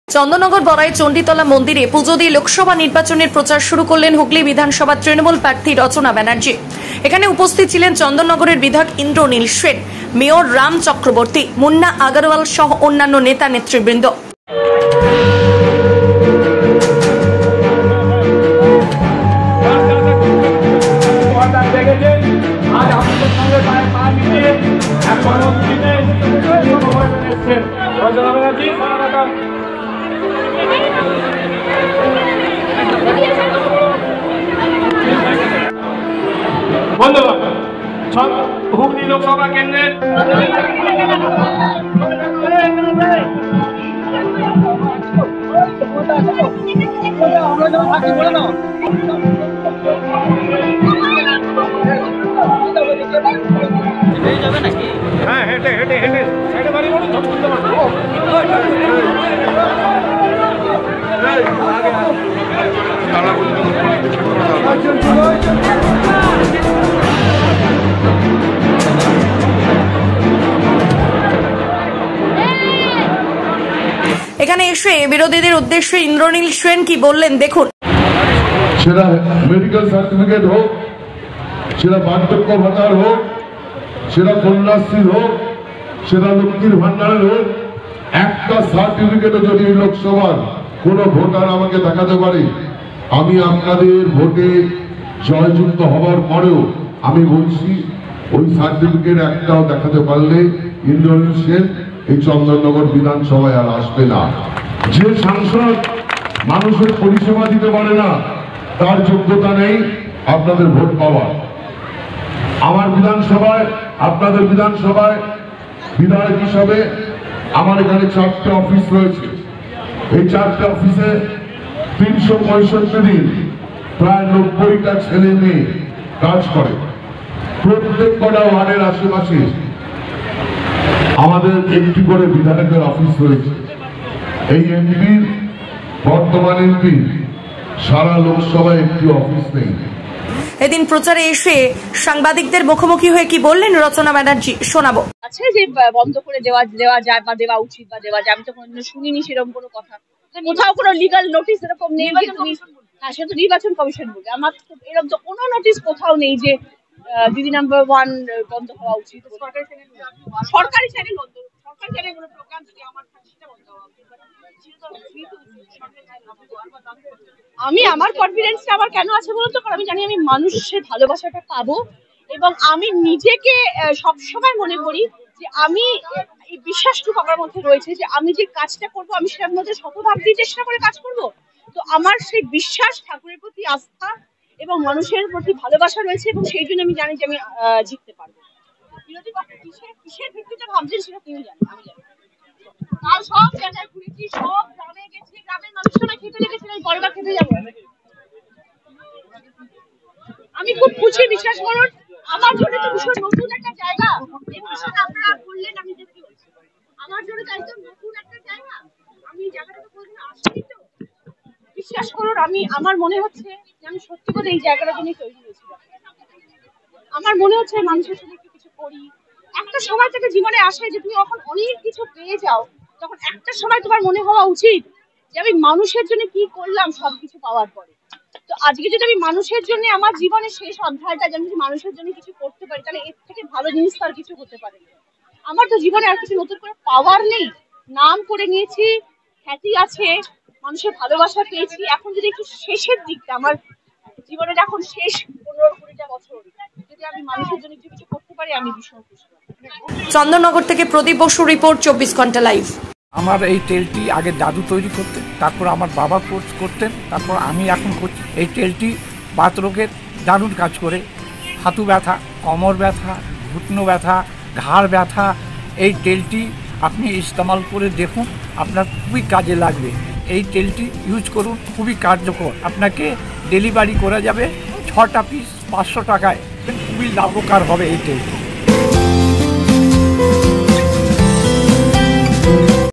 Hooghly News:হুগলিতে তৃণমূলের প্রার্থী হিসেবে এবার প্রচার শুরু করলেন রচনা বন্দ্যোপাধ্যায়
হুগলিতে তৃণমূলের প্রার্থী হিসেবে এবার প্রচার শুরু করলেন রচনা বন্দ্যোপাধ্যায়